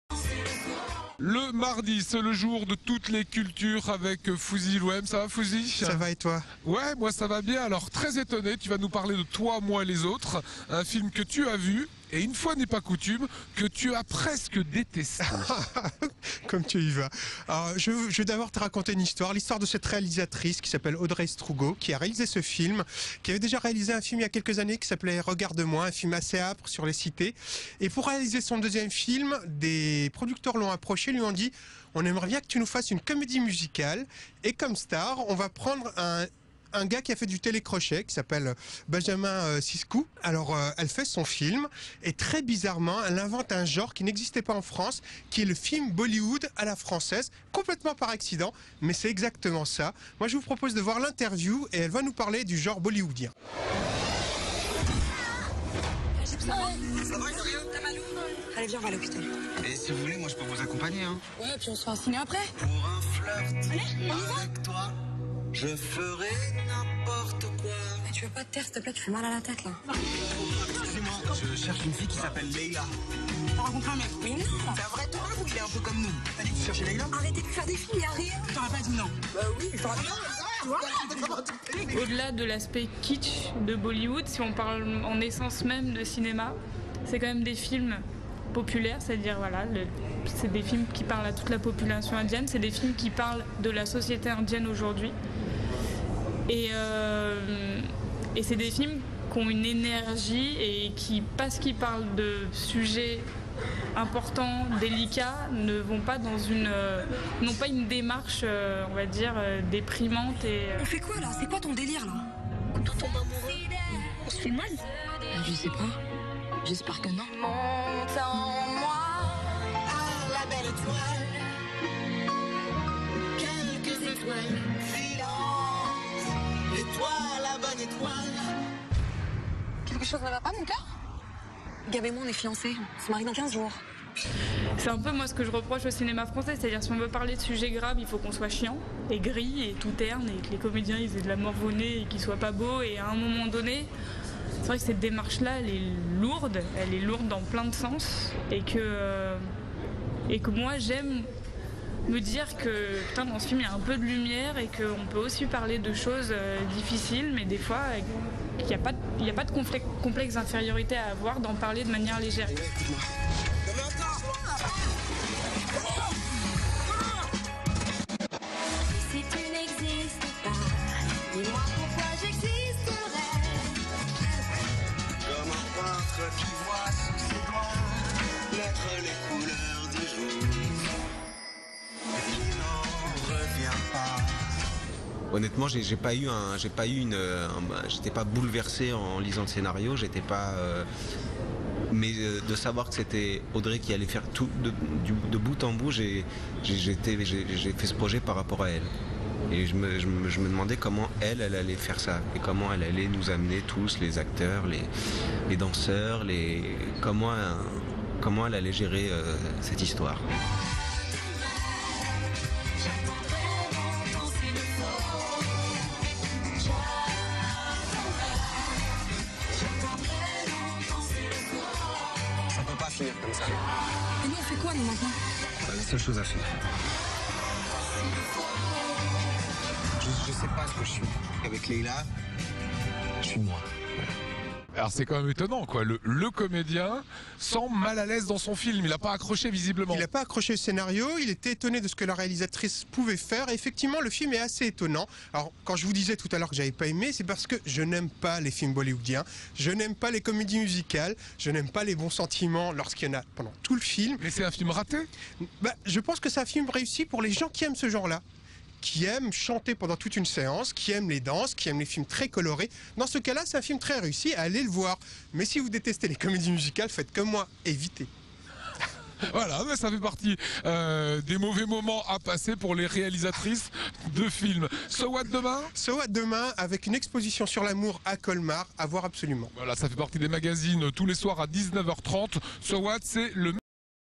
"Toi, moi, les autres": un bollywood à la française?
Vous voulez conserver ce reportage ?
 Focus sur le film "Toi, moi, les autres", réalisé par Audrey Estrougo.